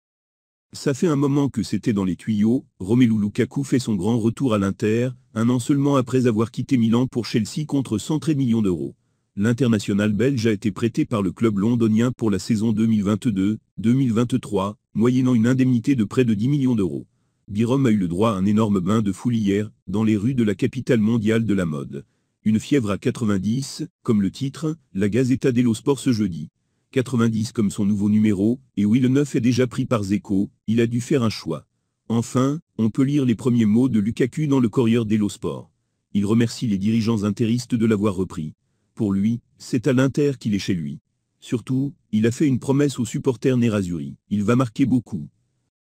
Inter Milan : Lukaku promet du lourd pour la saison prochaine
Ça fait un moment que c’était dans les tuyaux, Romelu Lukaku fait son grand retour à l’Inter, un an seulement après avoir quitté Milan pour Chelsea contre 113 millions d’euros.